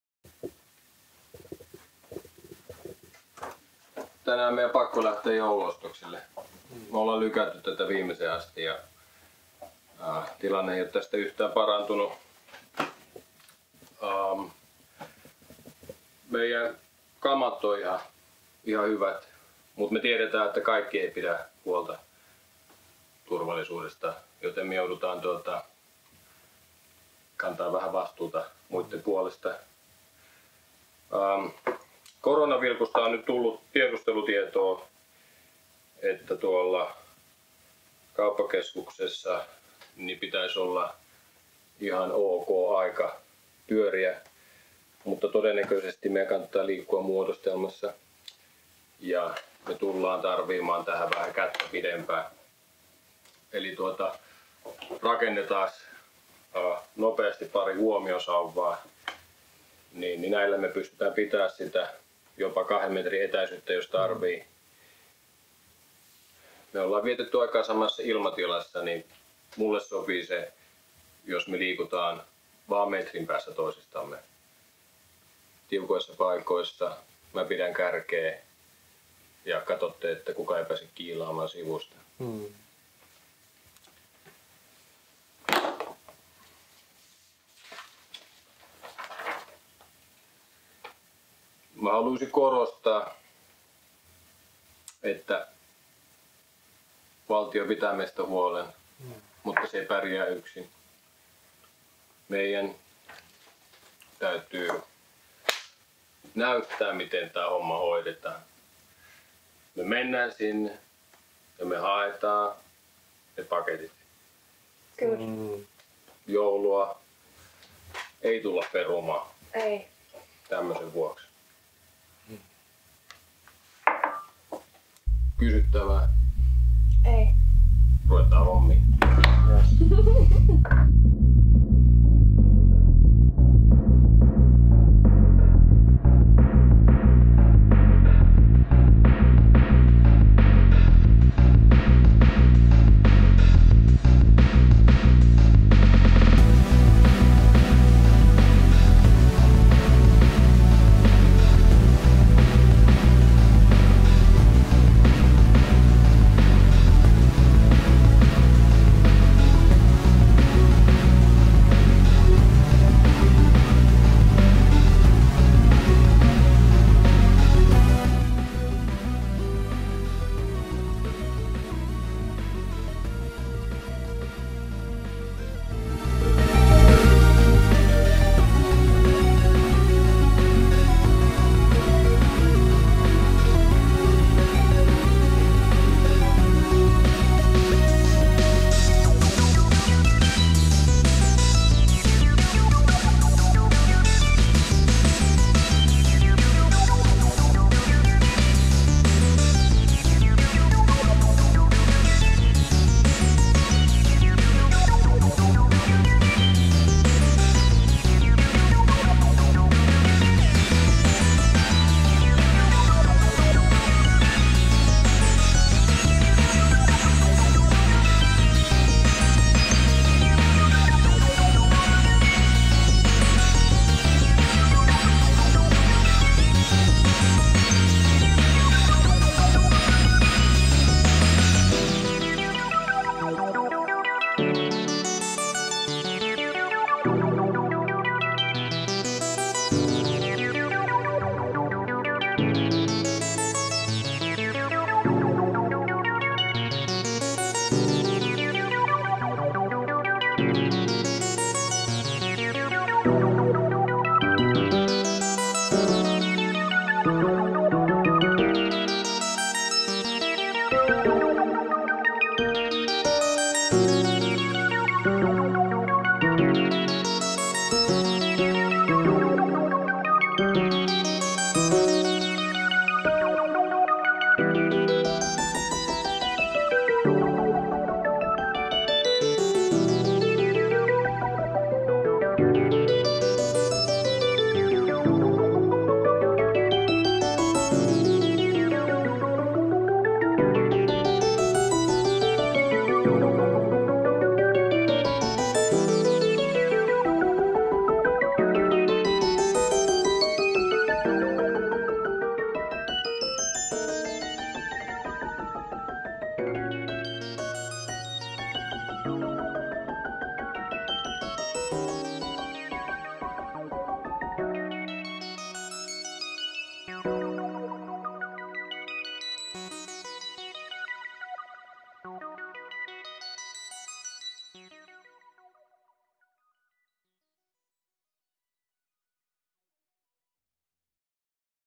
Jouluostoksilla turvataso 5
Käytännön vinkkejä joulushoppailuun, oikeilla varusteilla poikkeustilannetta ei juuri huomaakkaan.